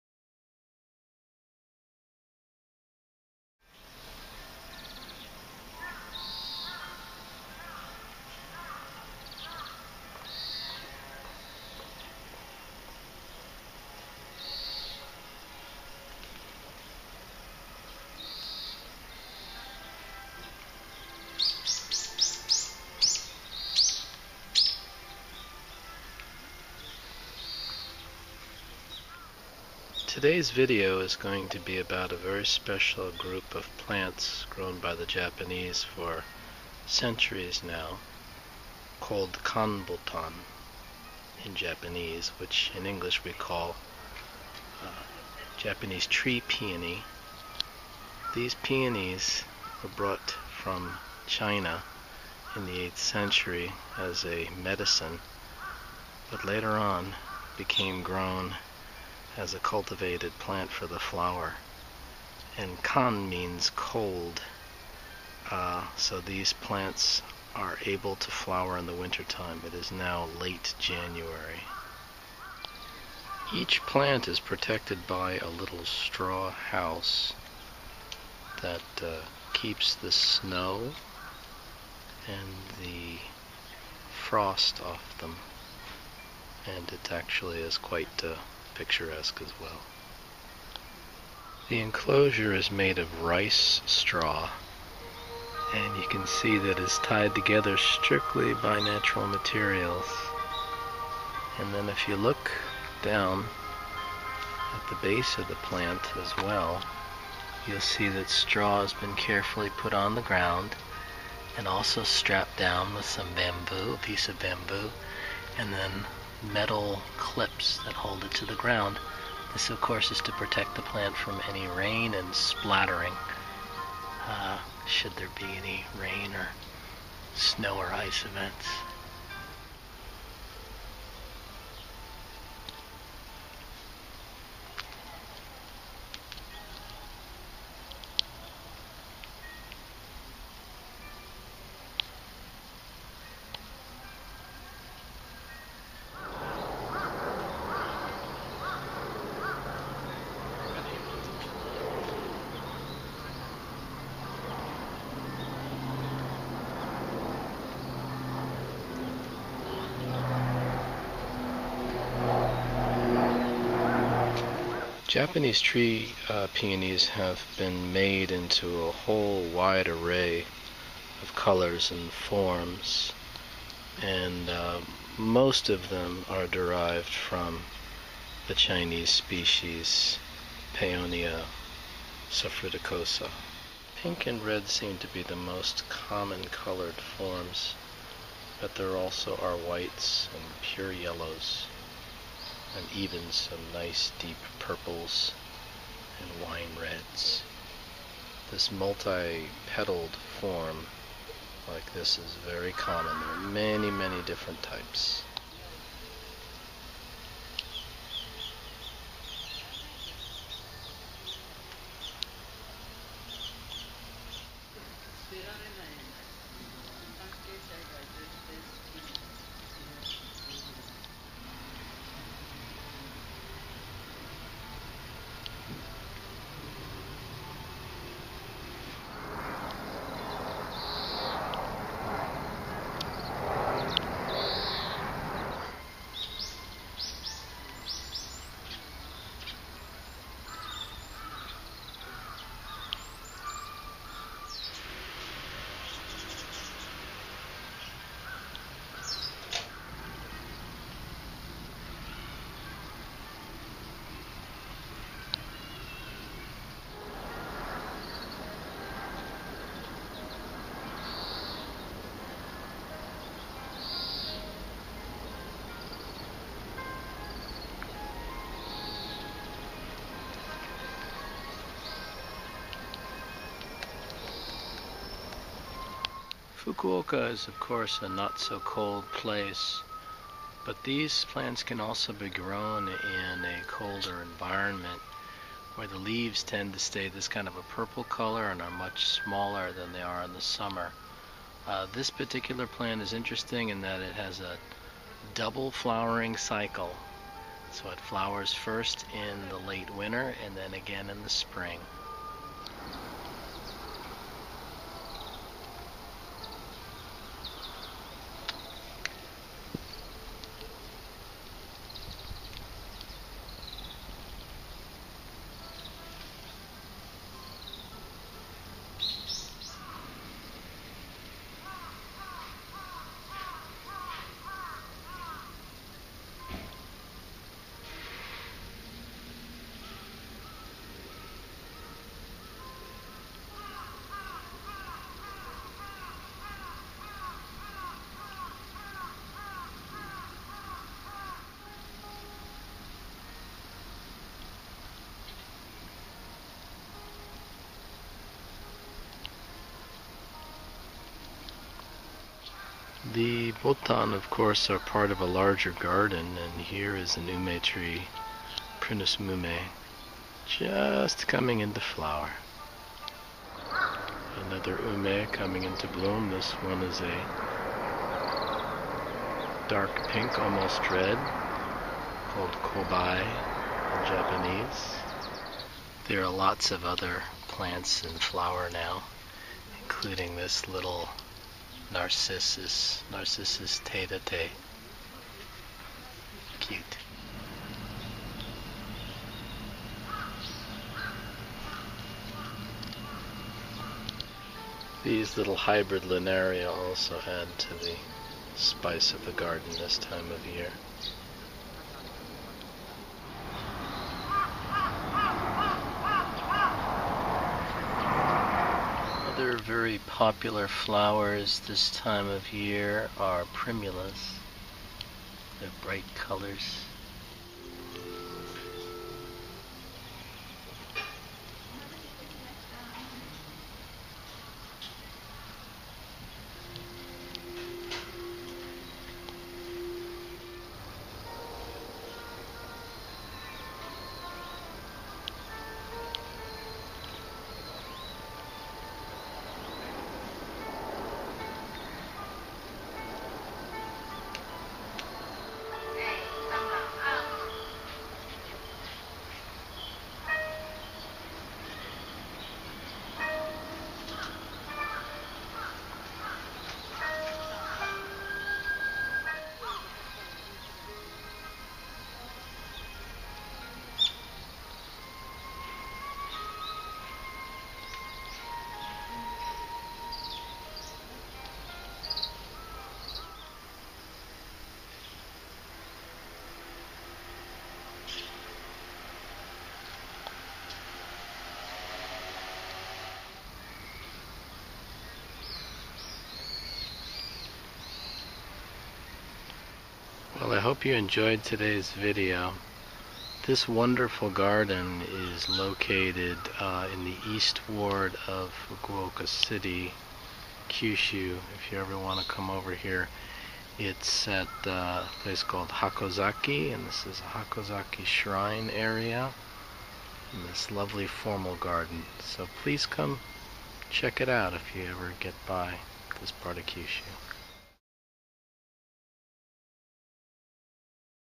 Japanese Tree Peonies in Winter: Hakozaki Shrine Flower Garden
For centuries both Japanese and Chinese gardeners have been growing tree peonies.  While there are several species of tree peony, Paeonia suffruticosa is the most commonly grown and has been bred to create a great variety of flower types.  In Japan plants have been selected for cold flowering as well, the so called Kanbotan, or cold flowering peonies.  These actually flower in mid winter and again in the spring.  This video focuses on this group of peony at a flower garden that is part of a Shinto Shrine, Hakozaki Shrine in Fukuoka City, Kyushu, Japan. You'll get to see how they are grown and also the surrounding gardens and companion plants on a winter's day.